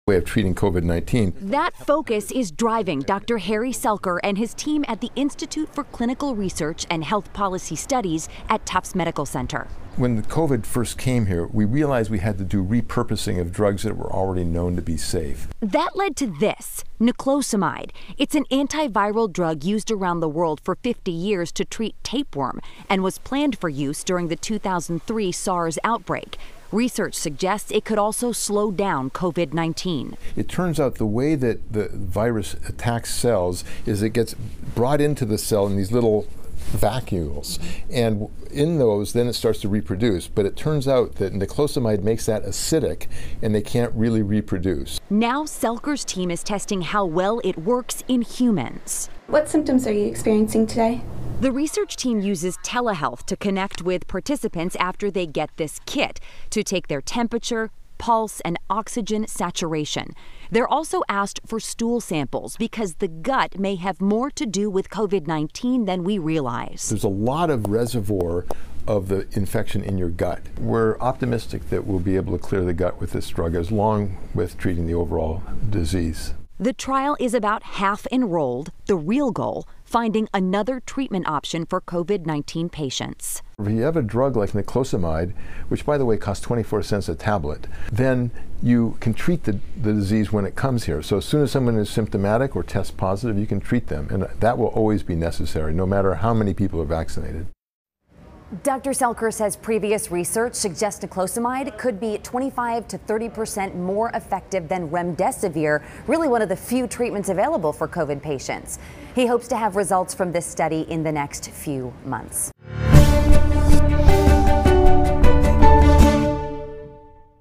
New England researchers testing Niclosamide in COVID-19 patients
A team at Tufts Medical Center and teams at five other New England hospitals are testing how well it works.